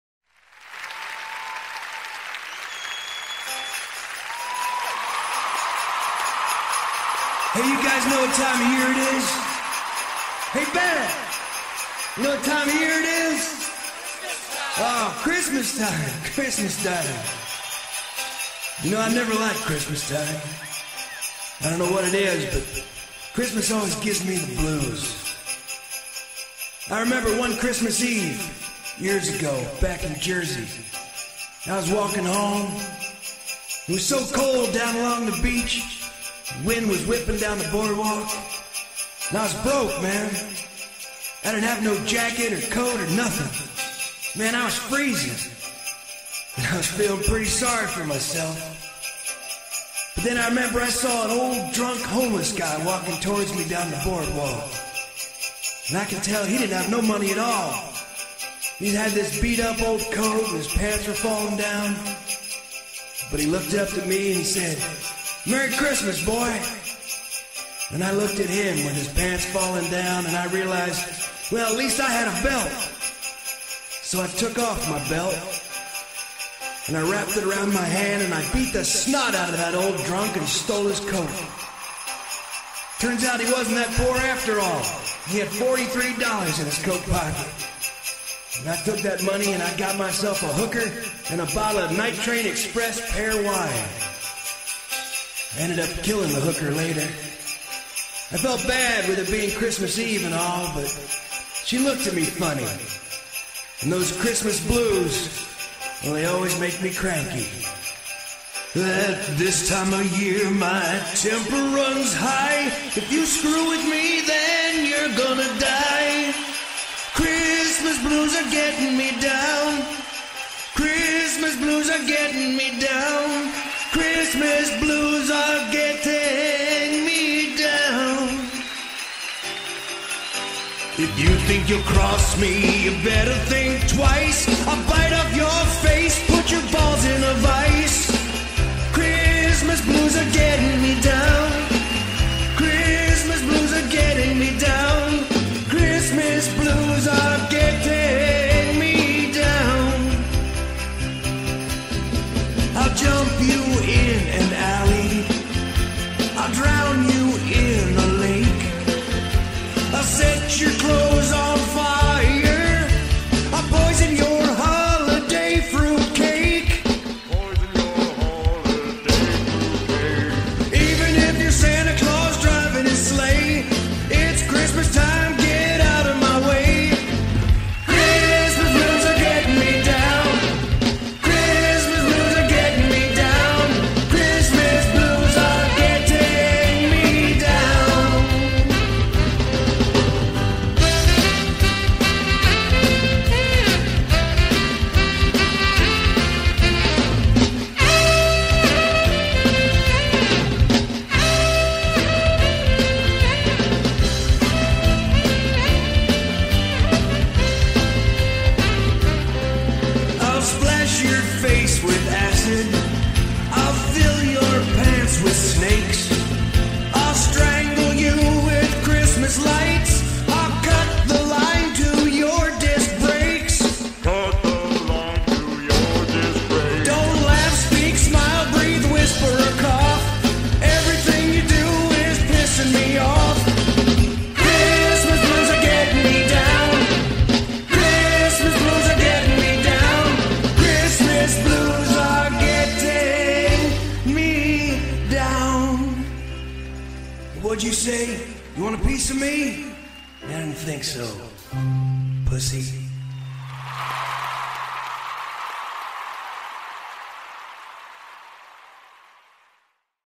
CHRISTMAS BLUES by The Boss
Sometimes the pressure of the holidays can make "The Boss" a little cranky ...